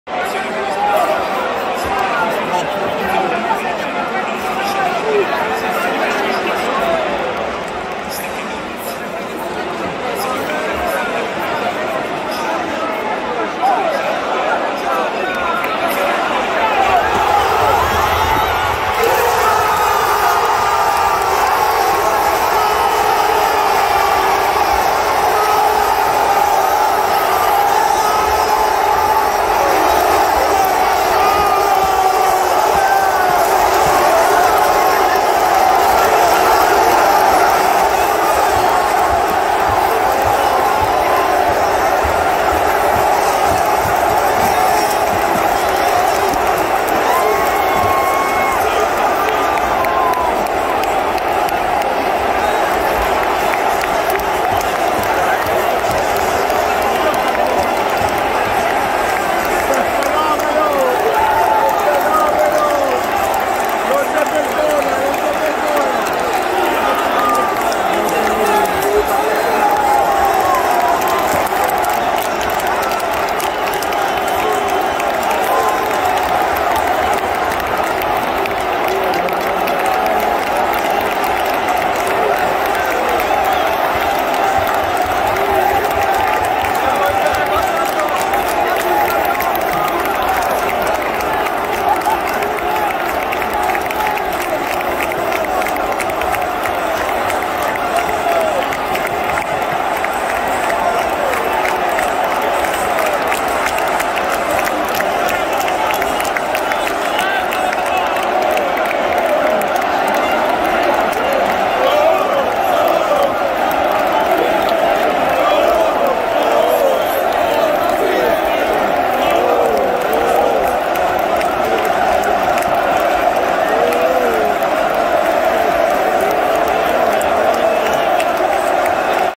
Dybala fa sognare l' Olimpico, poi la beffa ai rigori.
Finale di Europa League, il momentaneo vantaggio giallorosso fa letteralmente esplodere i 50mila accorsi allo stadio Olimpico davanti ai 6 maxischermi istallati per l'occasione.
Poi con la grande complicità dell'arbitro Taylor la partita più lunga della storia del calcio arriva ai calci di rigore.
Ibañez e Mancini falliscono e la Coppa viene alzata per la settima volta dal Siviglia.